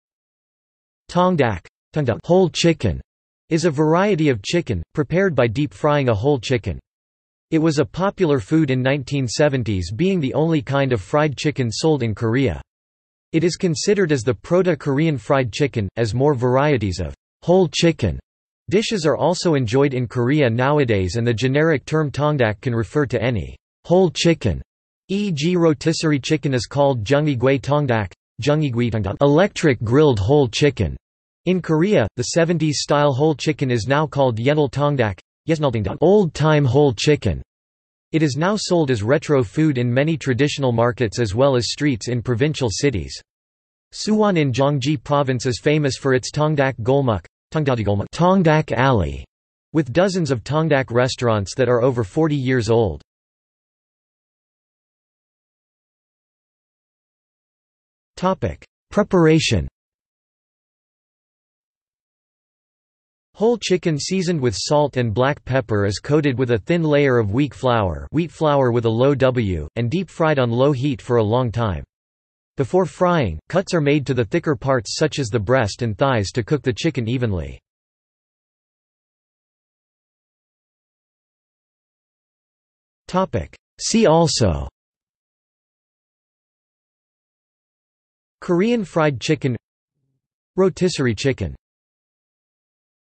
Tongdak | Wikipedia audio article
This is an audio version of the Wikipedia Article:
Tongdak

00:00:54 undefined
00:01:19 undefined

- Socrates

SUMMARY
Tongdak (통닭, "whole chicken") is a variety of chicken, prepared by deep-frying a whole chicken. It was a popular food in 1970s being the only kind of fried chicken sold in Korea. It is considered as the proto-Korean fried chicken.As more varieties of "whole chicken" dishes are also enjoyed in Korea nowadays and the generic term tongdak can refer to any "whole chicken" (e.g. rotisserie chicken is called jeongi-gui-tongdak (전기구이 통닭, "electric-grilled whole chicken") in Korea), the 70's-style whole chicken is now called yennal-tongdak (옛날통닭, "old-time whole chicken"). It is now sold as retro food in many traditional markets as well as streets in provincial cities. Suwon in Gyeonggi Province is famous for its tongdak golmok (통닭골목, "tongdak alley") with dozens of tongdak restaurants that are over 40 years old.